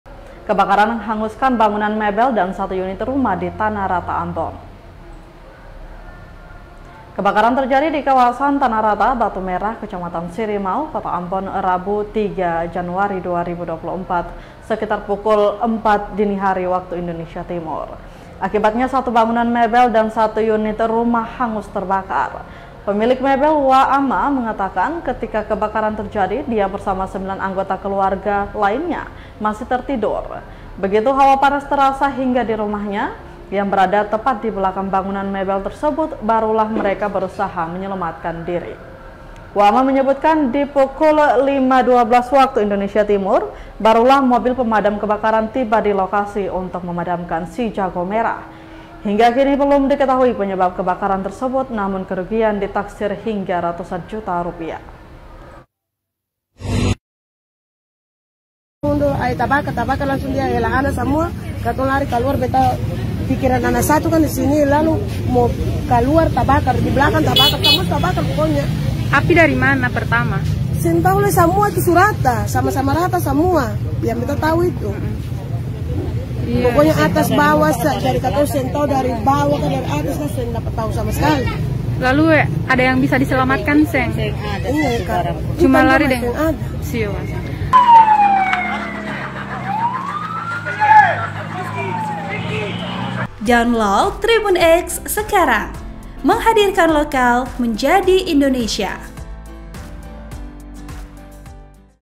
Kebakaran Hanguskan Bangunan Mebel dan 1 Unit Rumah di Tanah Rata – Ambon
OA,Laporan Wartawan TribunAmbon.com, Adjeng Hatalea
AMBON, TRIBUNAMBON.COM - Kebakaran terjadi di kawasan Tanah Rata, Batu Merah, Kecamatan Sirimau, Kota Ambon, Rabu (3/1/2023) sekitar pukul 04.00 WIT.
Akibatnya, satu bangunan mebel dan satu unit rumah hangus terbakar.
Pemilik Mebel, Wa Ama mengatakan, ketika kebakaran terjadi, dia bersama sembilan anggota keluarga lainnya masih tertidur.
Begitu hawa panas terasa hingga di rumahnya, yang berada tepat di belakang bangunan mebel tersebut barulah mereka berusaha menyelamatkan diri.